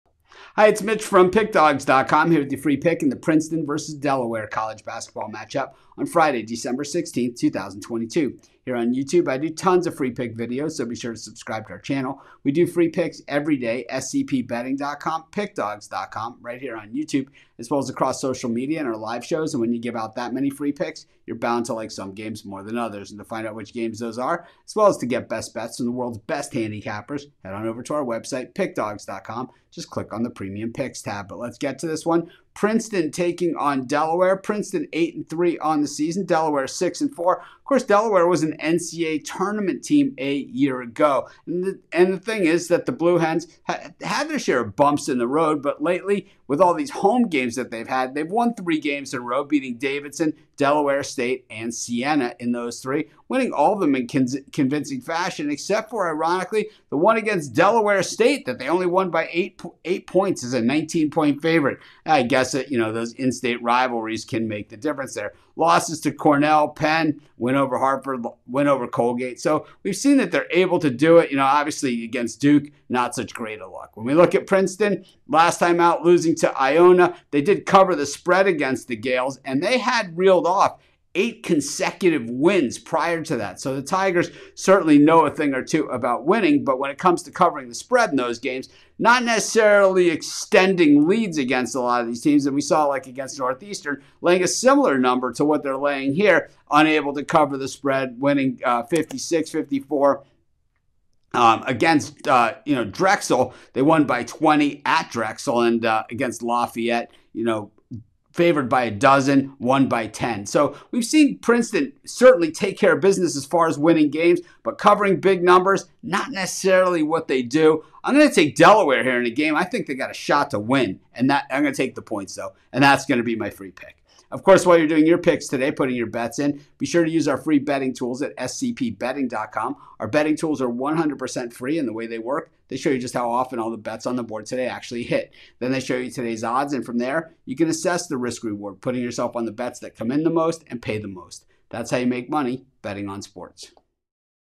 Princeton vs Delaware 12/16/22 College Basketball Free Pick CBB Betting Tips | NCAAB Picks
The Delaware Fightin' Blue Hens take on the Princeton Tigers in Wednesday College Basketball Action. Get Mitch's College Basketball Pick and Prediction for this game.